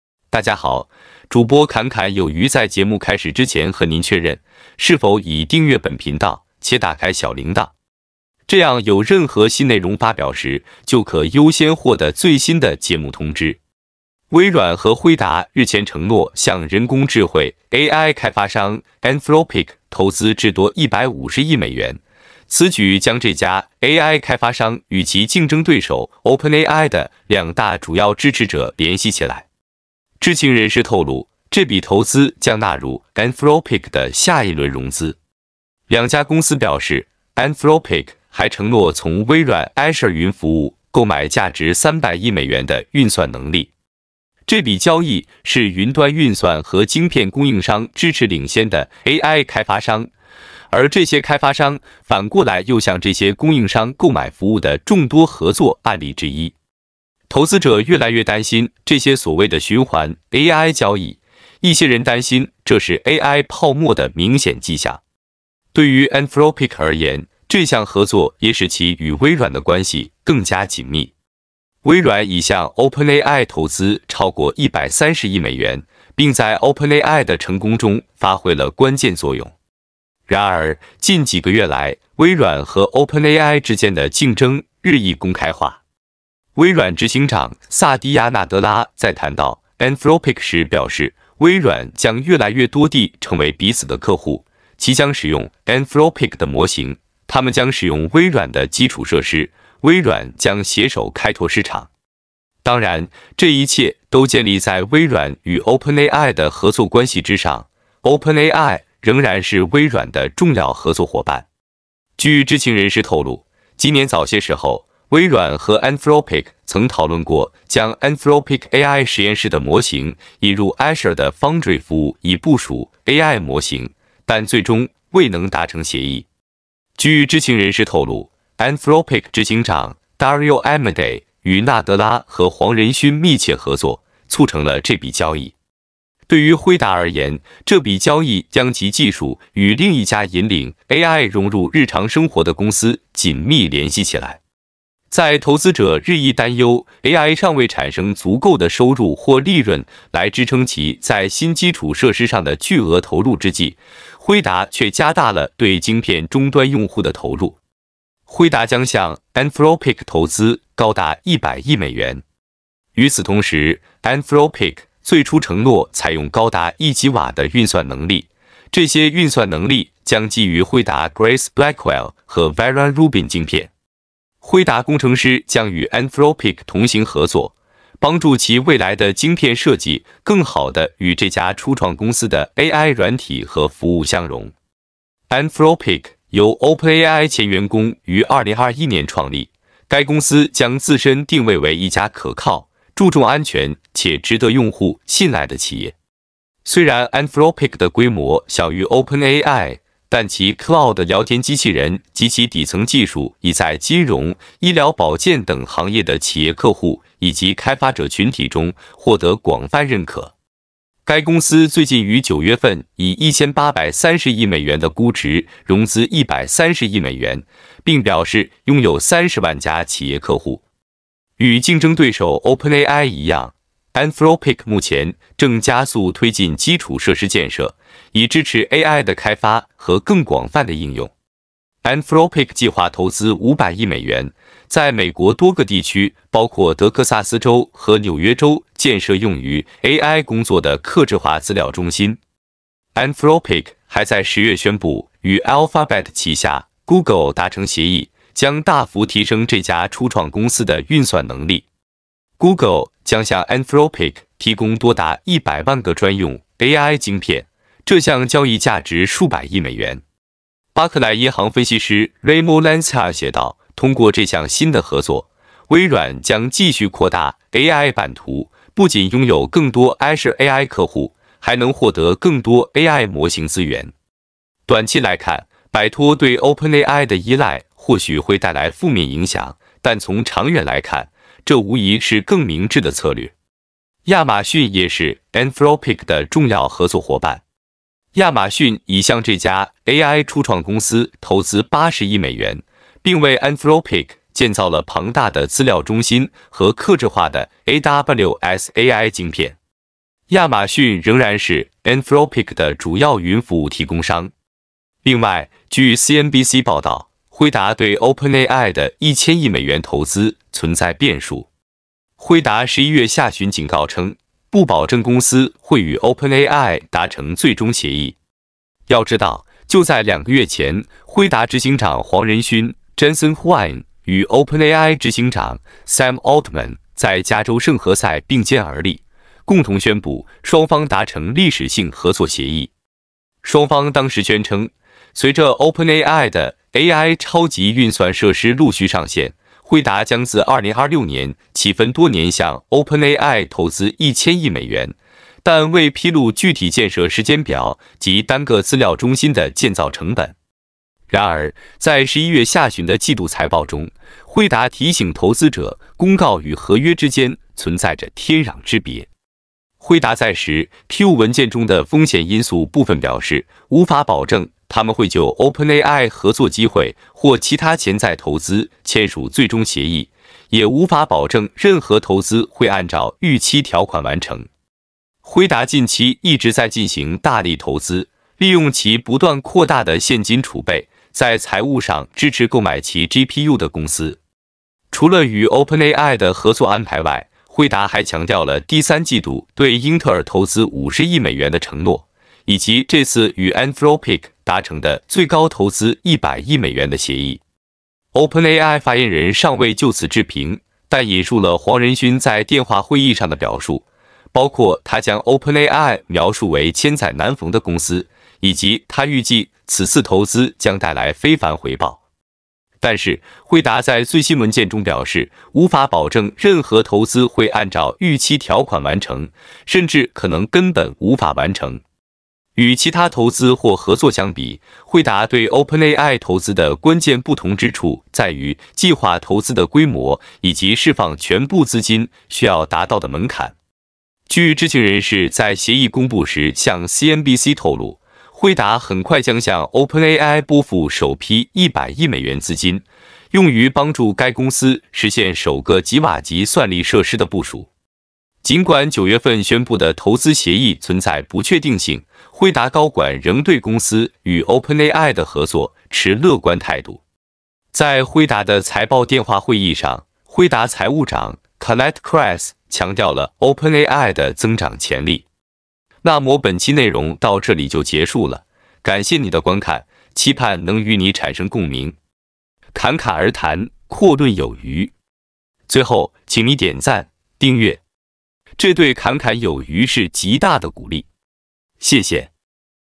慘烈泡湯！輝達反手徹底玩哭OpenAI，1000億美元投資極可能根本無法完成。轉而聯手微軟猛投AI創企Anthropic，運算能力基於Grace Blackwell和Vera Rubin晶片。
與競爭對手OpenAI一樣，Anthropic目前正加速推進基礎設施建設，以支持AI的開發和更廣泛的應用。輝達將向Anthropic投資高達100億美元。與此同時，Anthropic最初承諾採用高達1吉瓦的運算能力。據CNBC報道，輝達對OpenAI的1000億美元投資存在變數。輝達11月下旬警告稱，不保證公司會與OpenAI達成最終協議。